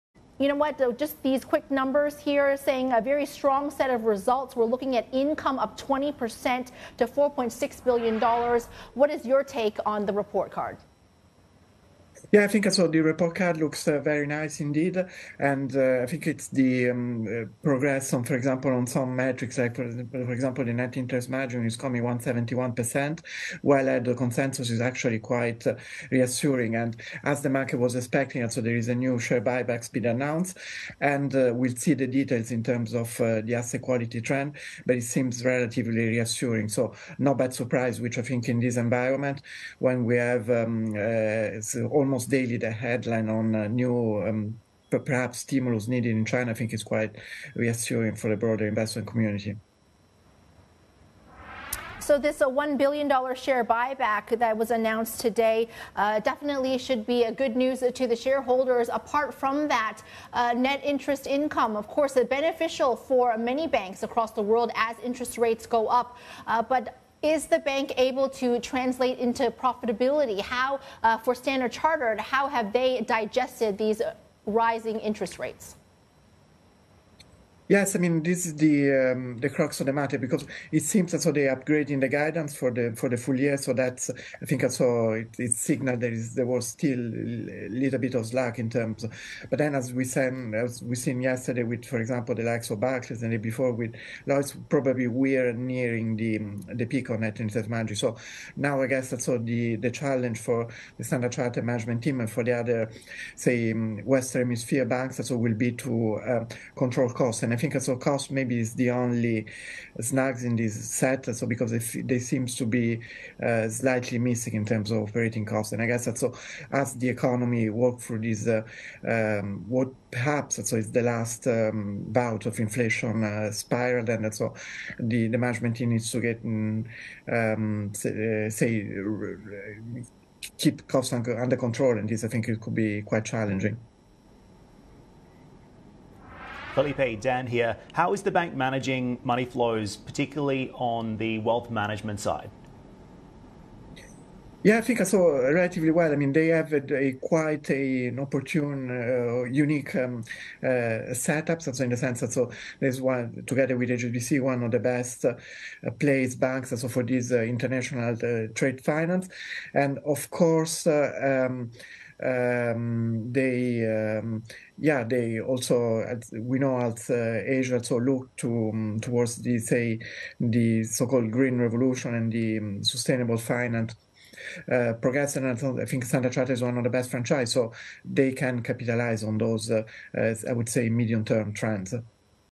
Strategist discusses Standard Chartered's earnings
Filippo Alloatti, senior credit analyst at Federated Hermes, discusses the bank's earnings report.